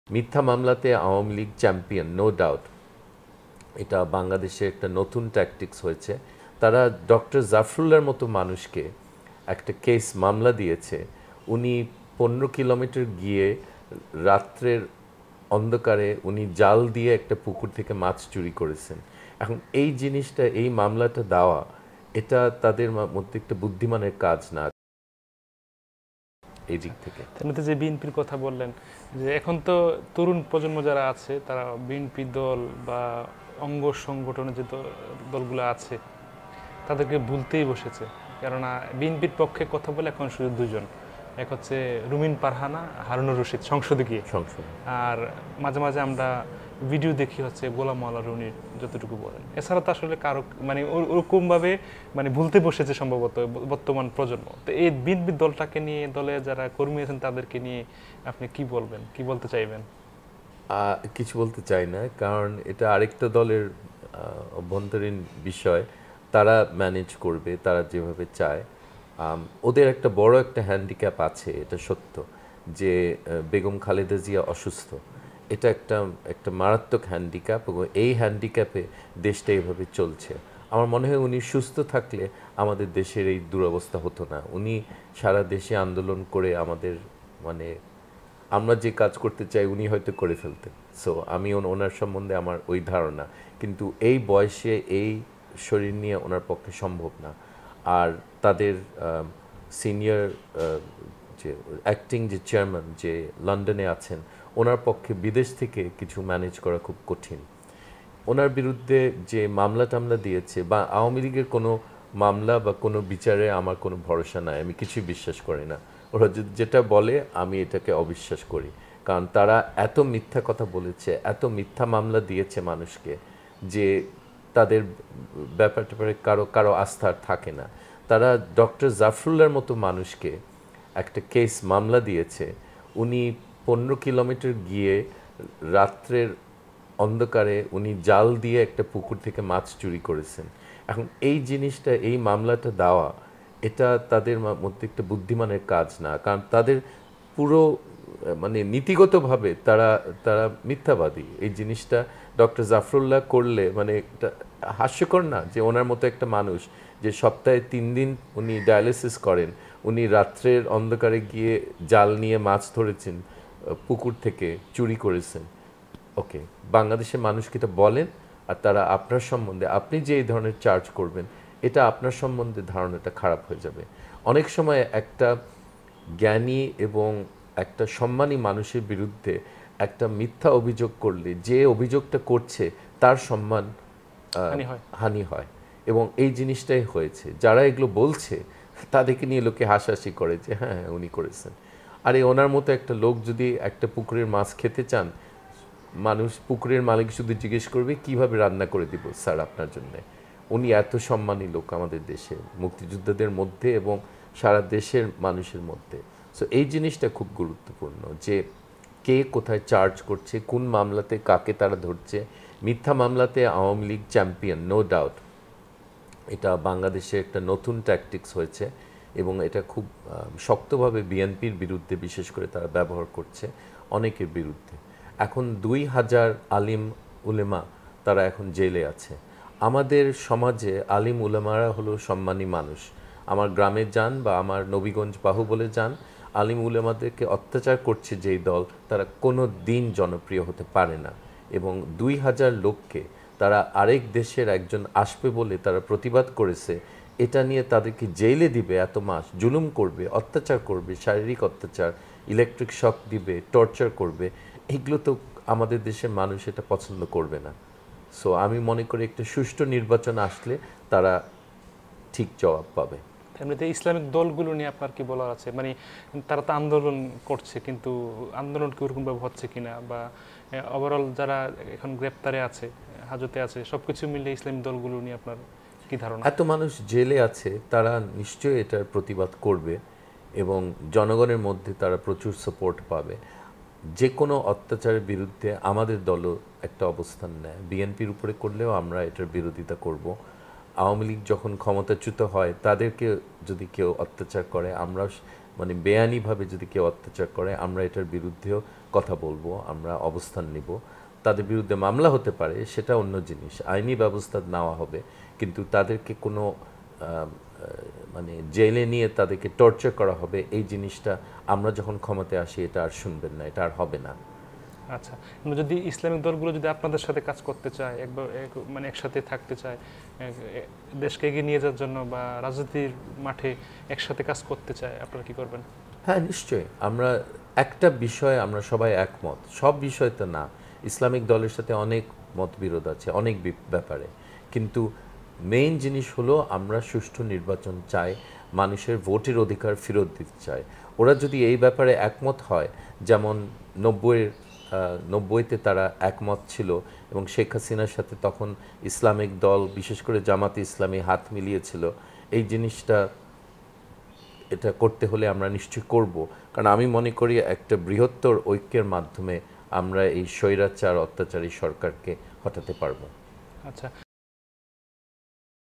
জামায়াত যদি নুরের দলে জোট করে । জাফরুল্লাহ মাছ চুরি করেছেন! রেজা কিবরিয়া-Chithi
Please Read...

MD.SAYADUL HASAN
(Hasan Sayadul)

আল্লাহ...